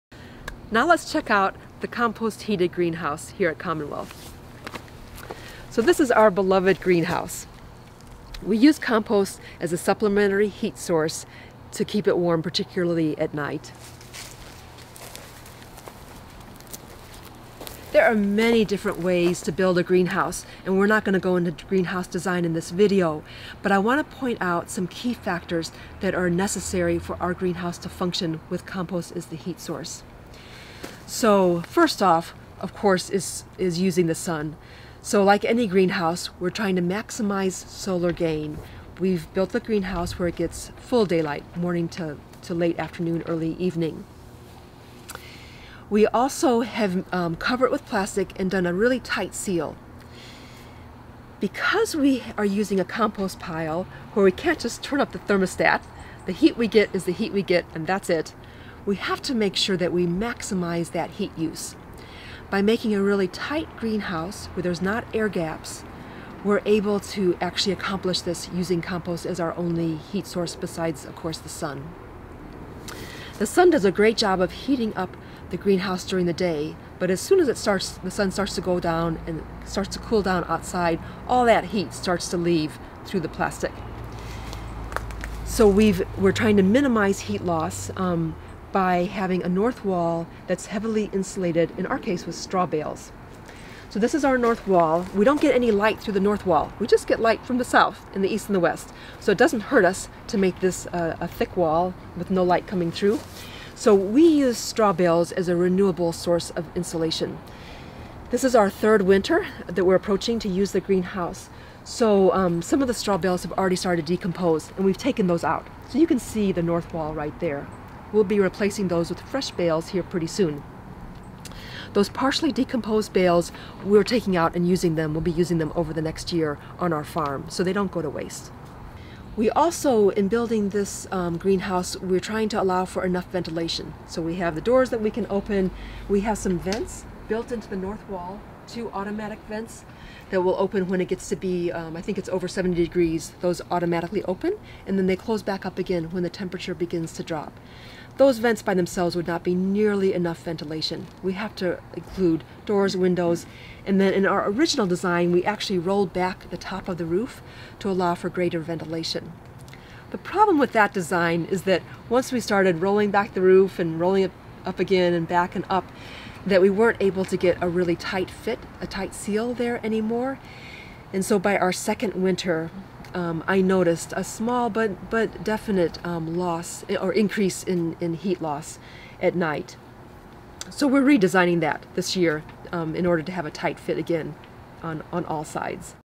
Compost as a Heat Source -Episode #5: Elements of the CommonWealth Greenhouse in 2021
Sun, insulation, ventilation & other critical elements.